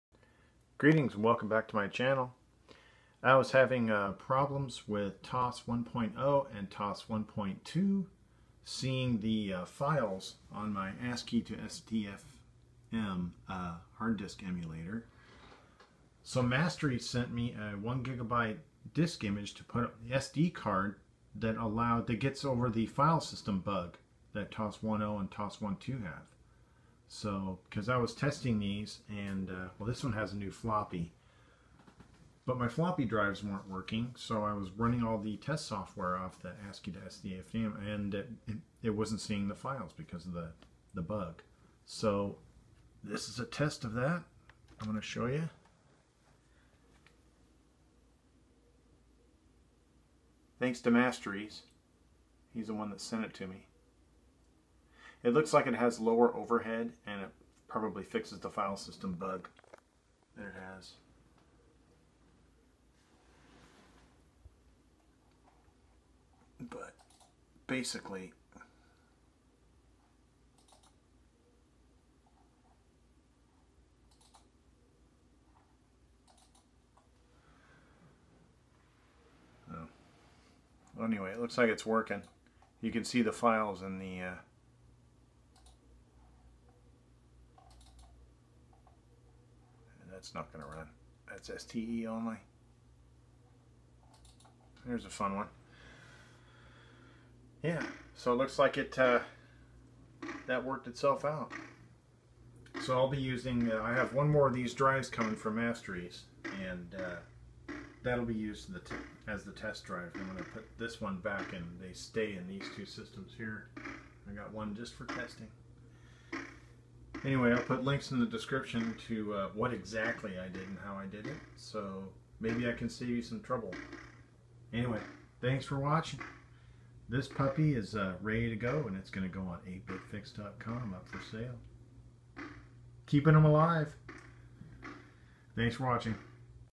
Atari 1040ST - ASC2STF - TOS 1.0, 1.2 Bug Fix - Special 1GB Disk Image Test -
In this video, I show how the special disk image I recieved from masteries works.
TOS 1.0 and 1.2 have problems with the fat 16 file system hard drives use.
Masteries ( of Metal Slug Demo Fame ) sent me a special 1gb disk image I burned to a SD card with Win32 Disk Imager. In TOS 1.0 and 1.2, when I would open a folder, there would either be nothing in it or there would be garbled characters you can't open. This fixes that problem. It was also nice to see the desktop start up in medium resolution - nice touch! There is no Gembench on this image, but there IS Sysinfo.

*The only caveat is that Windows 10 does not see the image, so you can't write to it, as in, add games and such * BUT *- so far all the games I have tried on this image have worked and there is a shit ton of good games on it.

I had eleven ST systems to recap then test and my external floppy drives were not working, so I had to use the ASC2ST device to load Gembench and Sysinfo, and the bug prevented me from doing so.

Thanks to Peter Putnik ( Paranoid Little Man )for developing this, and Masteries for sending it to me !
As well as hand making these drives , He is also an awesome programmer on the STe

You can get the ASC2STF Here :

" New Atari ST/E Mega ST/E hard disk interface "

Visit Eightbitfix.com